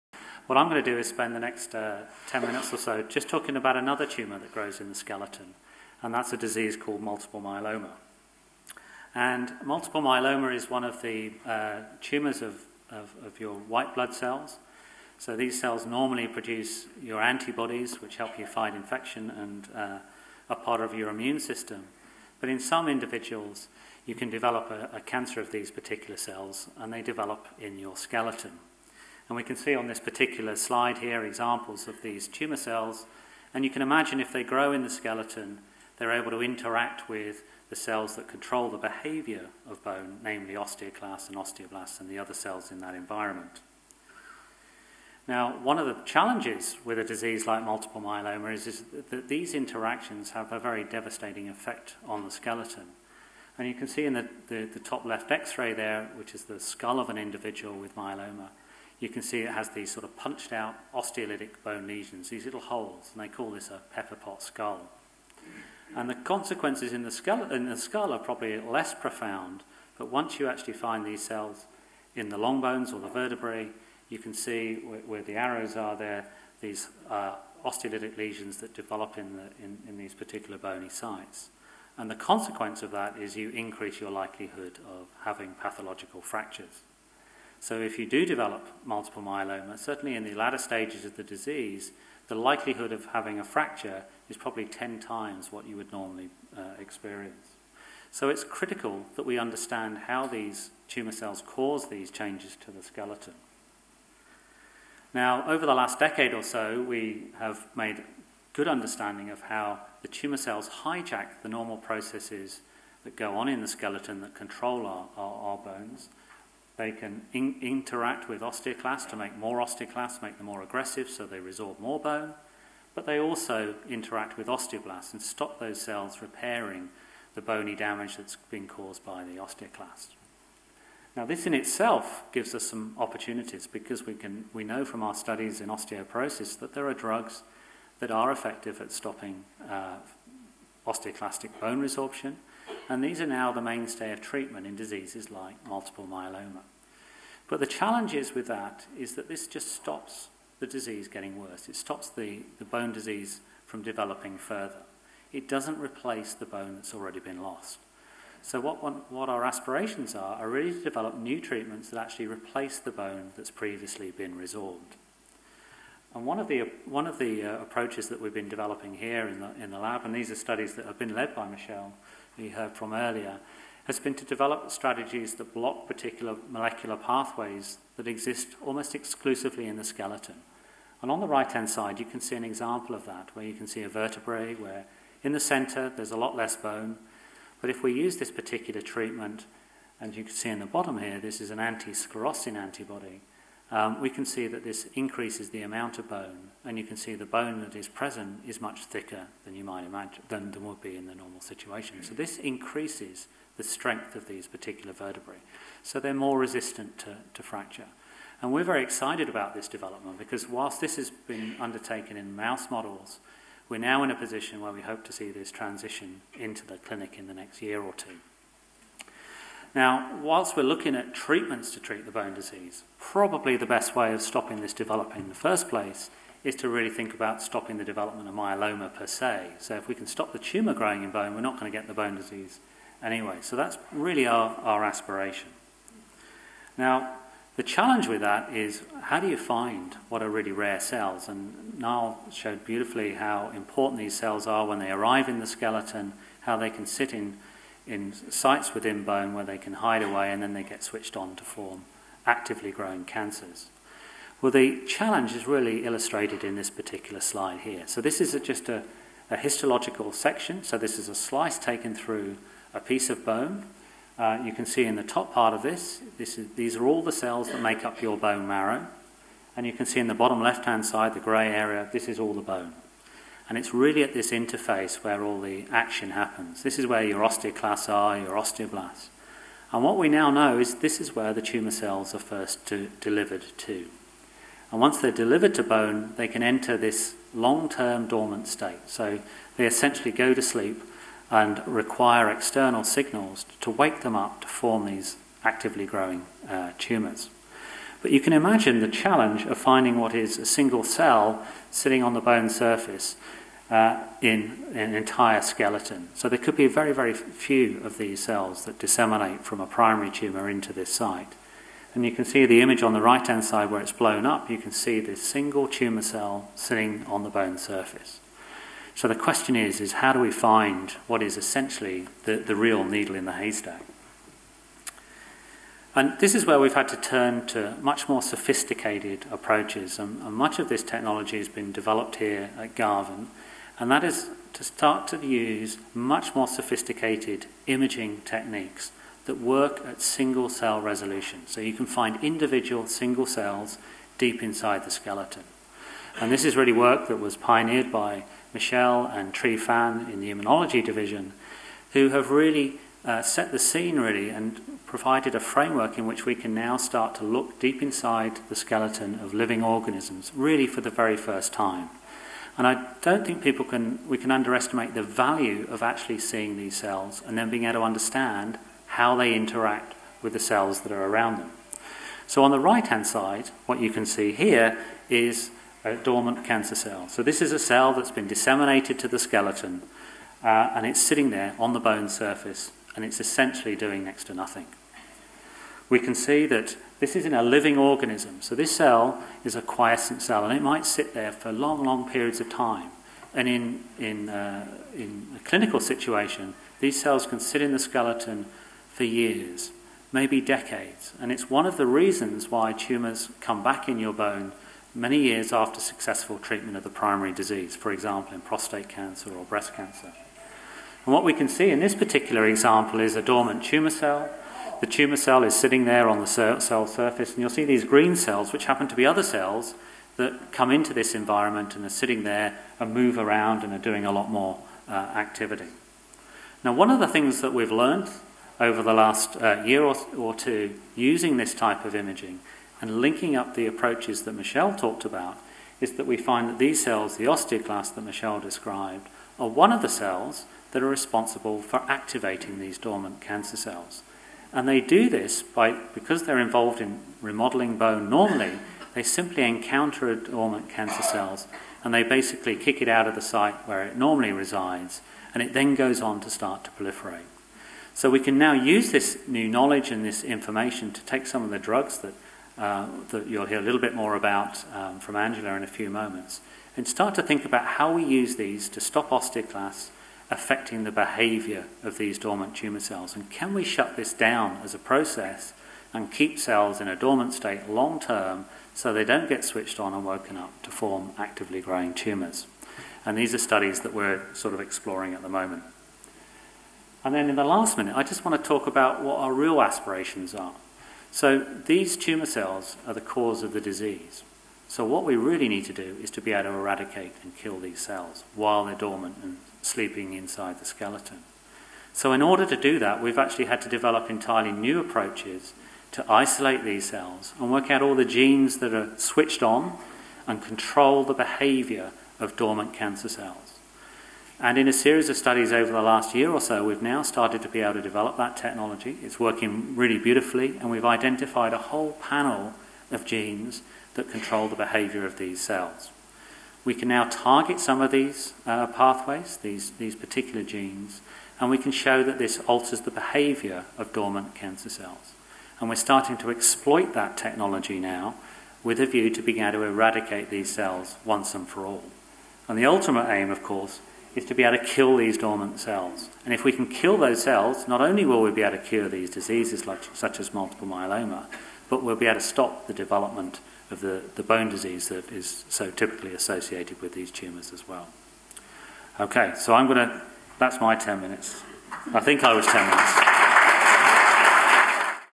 Solving the biggest mysteries: multiple myeloma and cell dormancy - Prof Peter Croucher
In this presentation, Professor Peter Croucher looks at Multiple Myeloma, a cancer of antibody producing blood cells that develops in the bone.